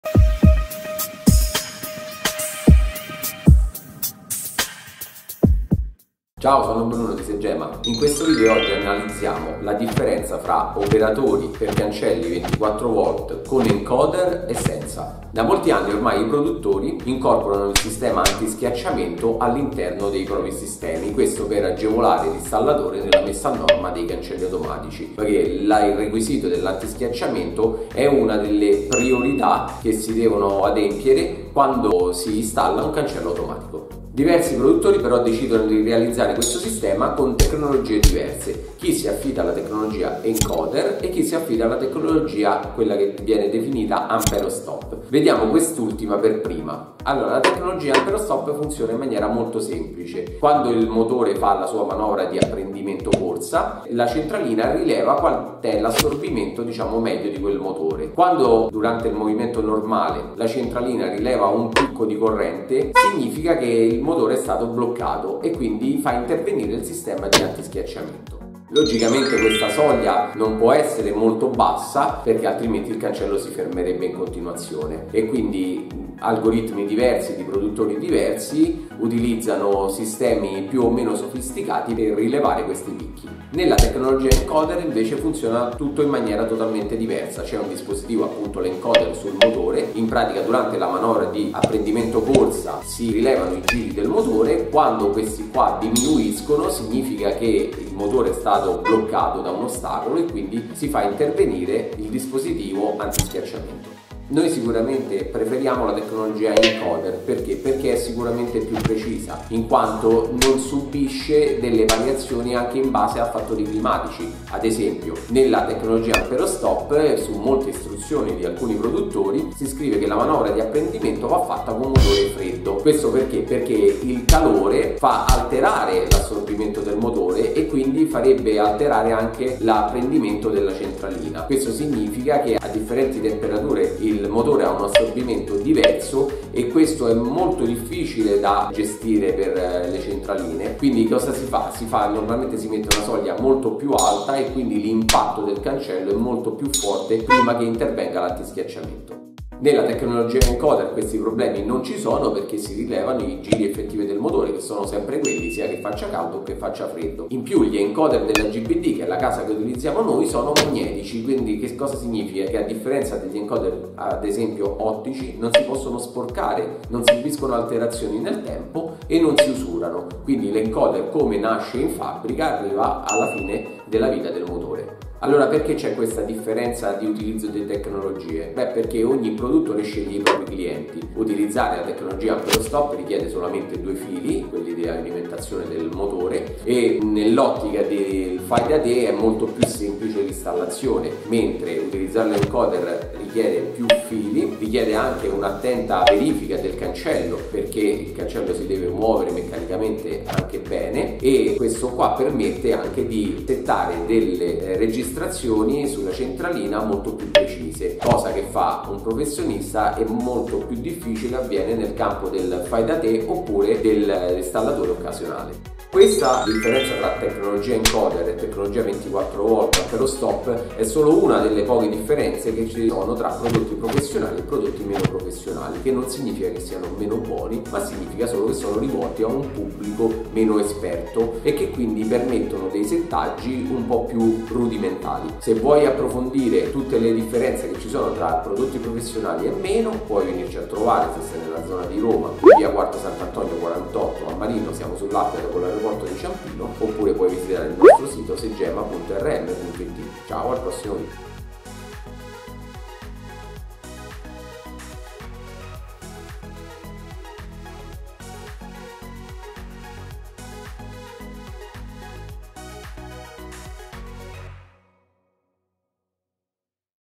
Differenze tra motori 24v con encoder e senza
In questo video esaminiamo insieme le differenze tra operatori 24 V con tecnologia encoder e quelli a tecnologia amperostop. Ne vedremo le caratteristiche e quali sono adatti per installatori esperti e quali invece per il fai-da-te.

E come sempre...per favore, aiutaci a crescere!
Se il video ti è stato utile:
- clicca sulla campanella per rimanere aggiornato su tutti i nostri video
Grazie!